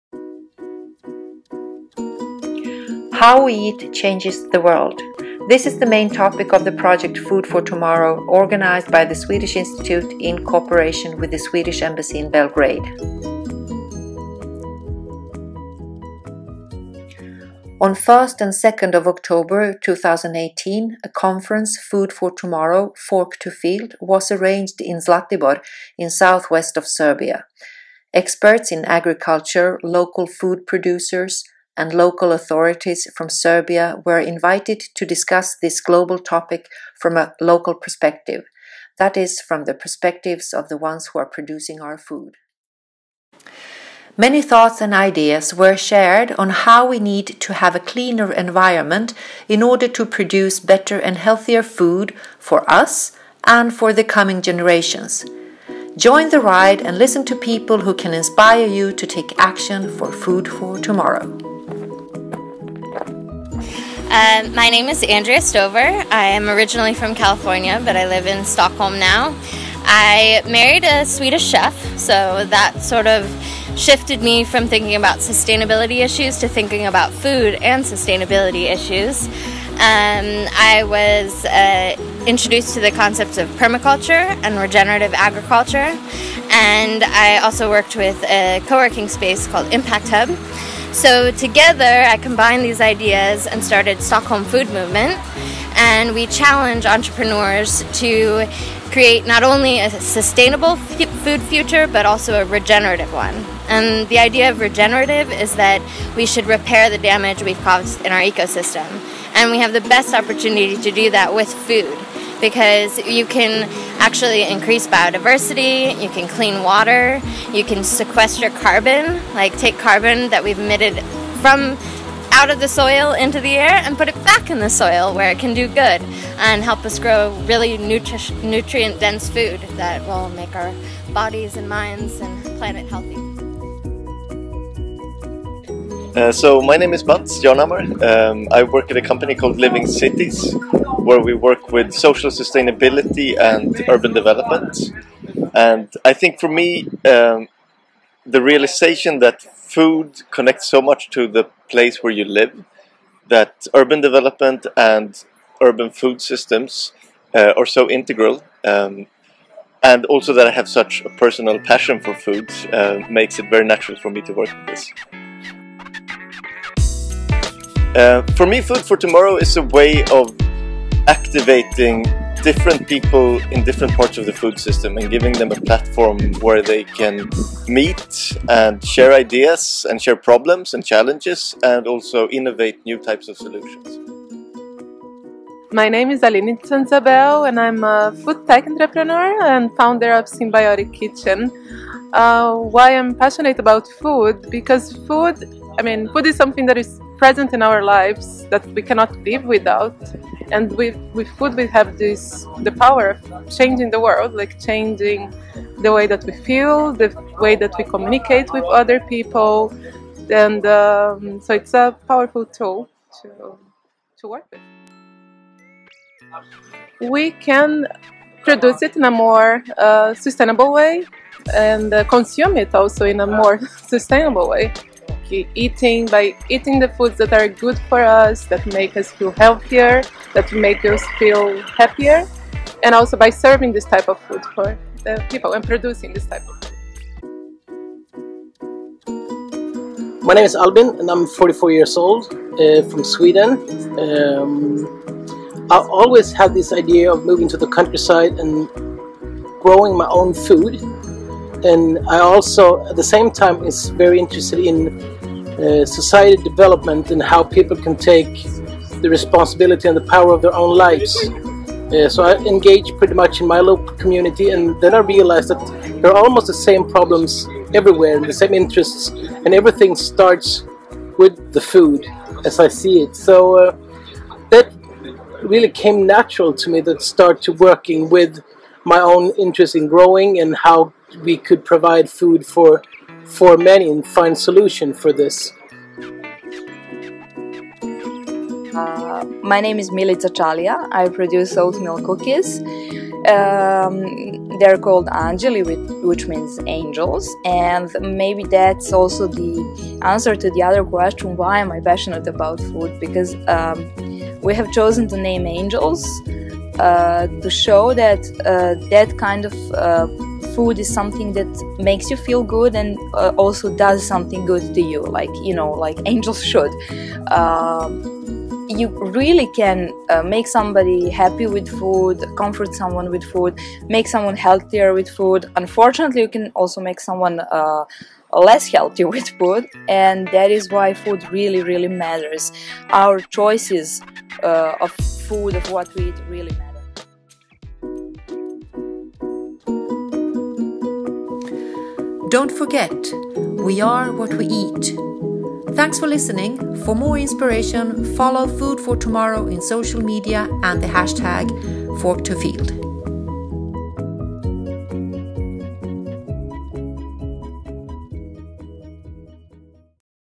Food for tomorrow-Zlatibor
What we eat changes the world, this was the topic of the project Food for tomorrow- fortofield, which was organized by the Embassy of Sweden in cooperation with the Swedish Institute. Take part of inspiring thoughts and the conference through this podcast.